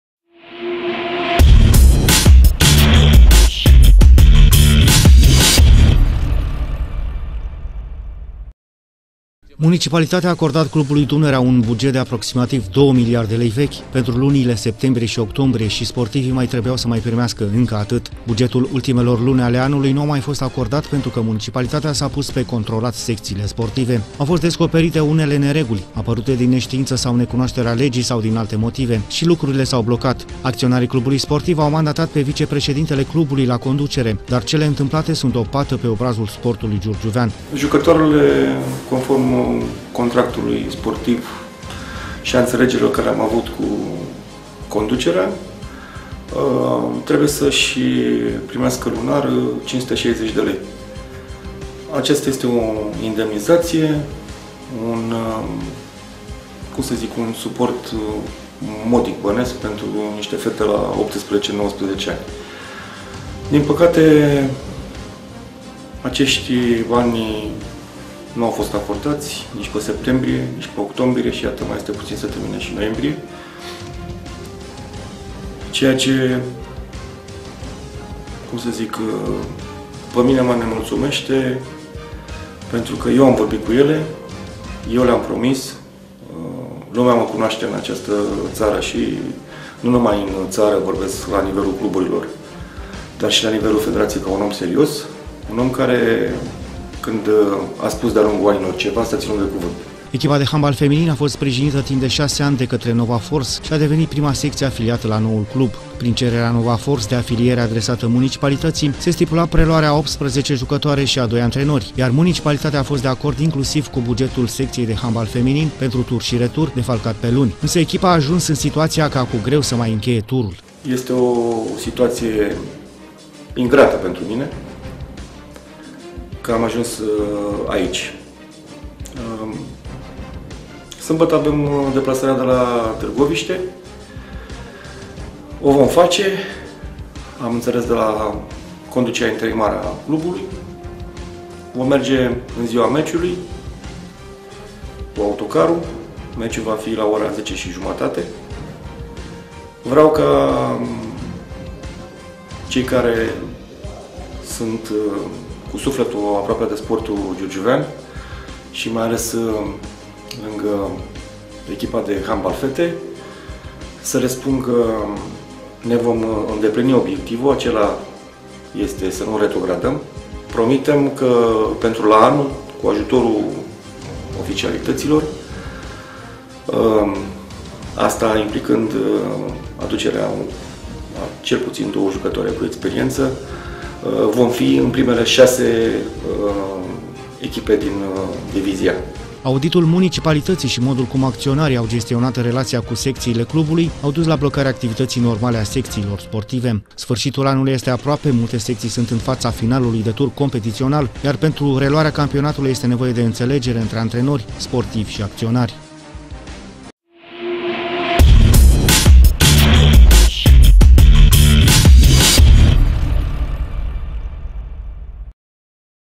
BLOCAJ LA CSM DUNAREA
Auditul comandat de municipalitate la CSM Dunărea Giurgiu a îngreunat activitatea mai multor secții sportive, iar unii antrenori au trebuit să suporte cheltuielile din buzunarul propriu. Imaginea antrenorilor și sportivilor are de suferit într-un moment în care sportul giurgiuvean trebuie finanțat, îndrumat și sprijinit.